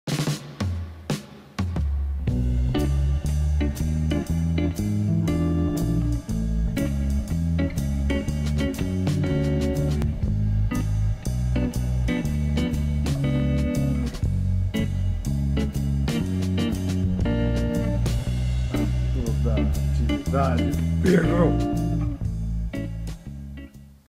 Igor the hyperactive Doberman
Igor doing what he loves the most, absolutely nothing.